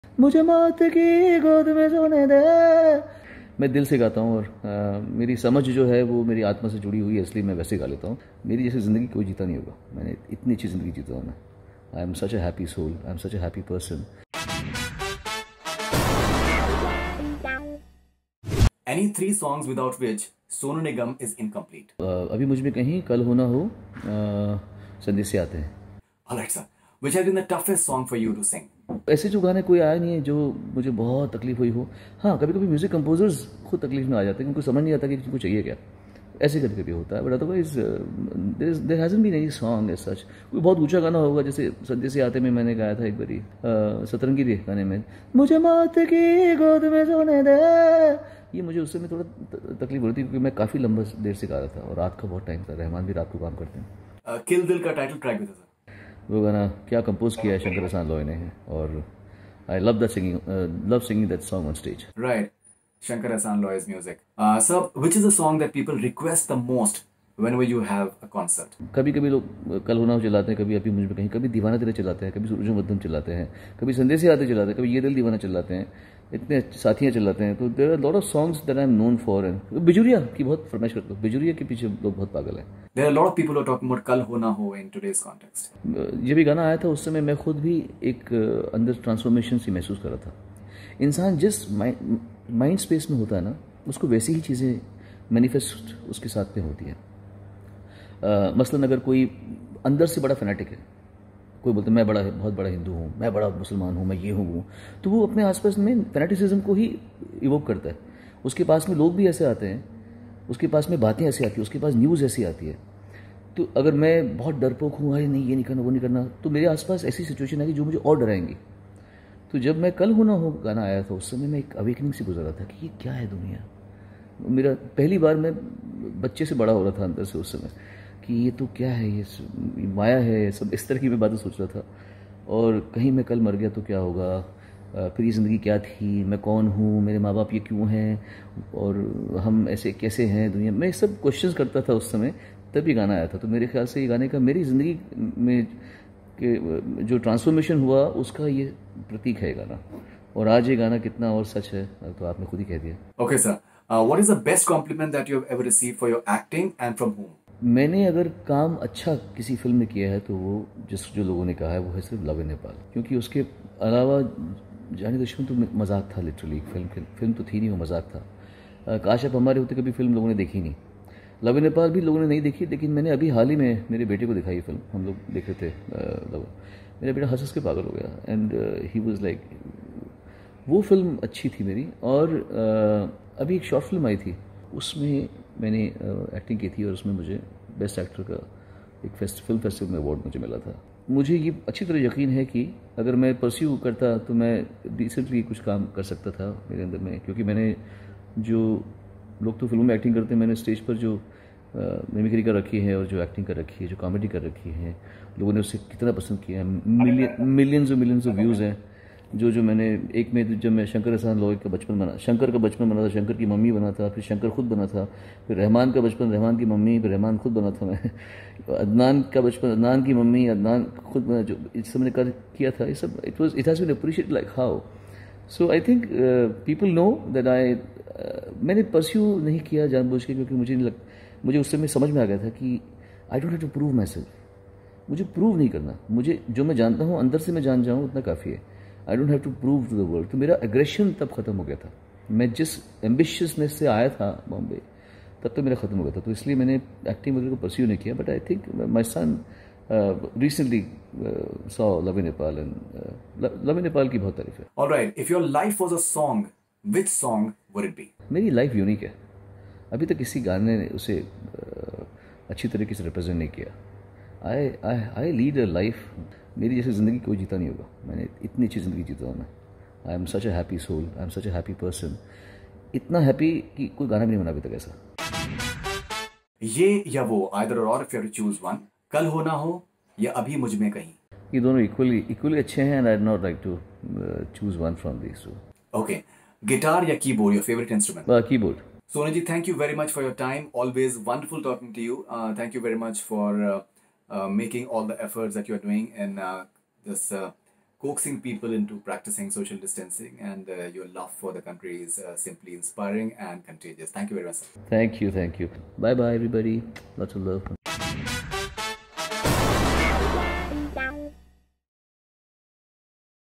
WOW- Sonu Nigam REVEALS 3 Songs without which he is INCOMPLETE | Rapid Fire
SonuNigam in an exclusive interview with Bollywood Hungama’s Faridoon Shahryar plays a super fun rapid fire and answers some interesting questions about his songs, his film career, 3 songs he's incomplete without etc. Don't miss!